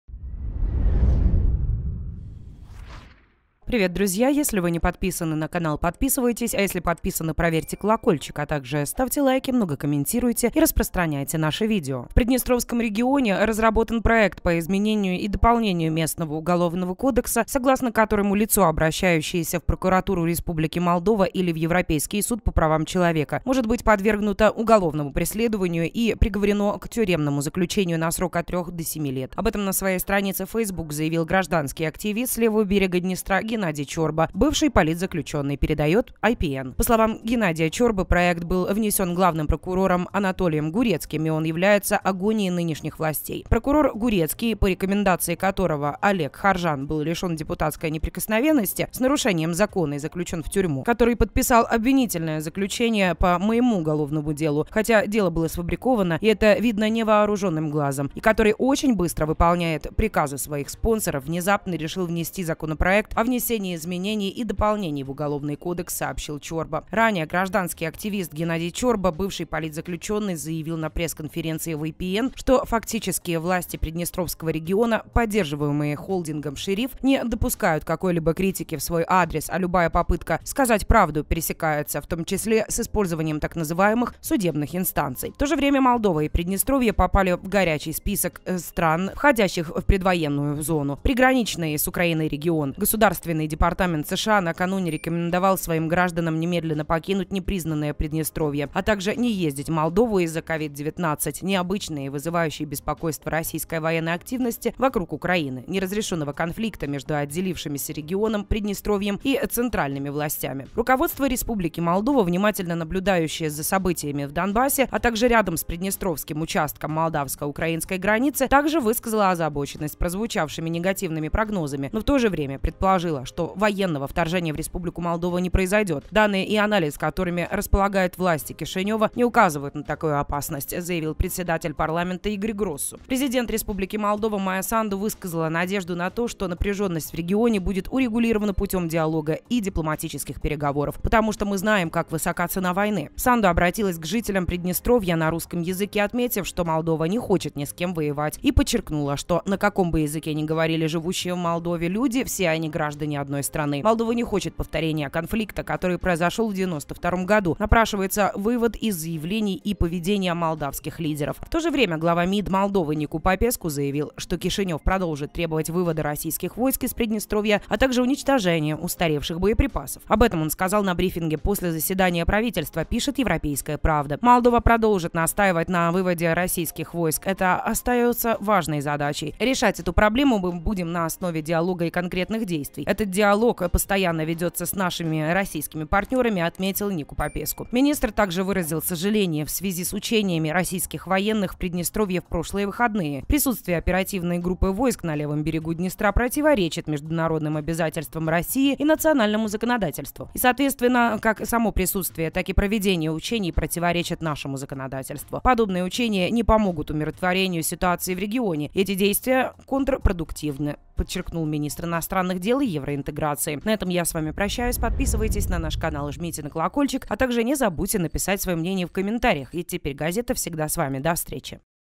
Срочное решение! Приднестровье идёт ва-банк –удар по Молдове: у Санду ответили. Ситуация накаляется!
Вот о чём идёт речь:

В приднестровском регионе разработан проект по изменению и дополнению местного Уголовного кодекса, согласно которому лицо, обращающееся в прокуратуру Республики Молдова или в Европейский суд по правам человека, может быть подвергнуто уголовному преследованию и приговорено к тюремному заключению на срок от трёх до семи лет. Об этом на своей странице в Facebook заявил гражданский активист с левого берега Днестра Геннадий Чорба, бывший политзаключённый, передаёт IPN.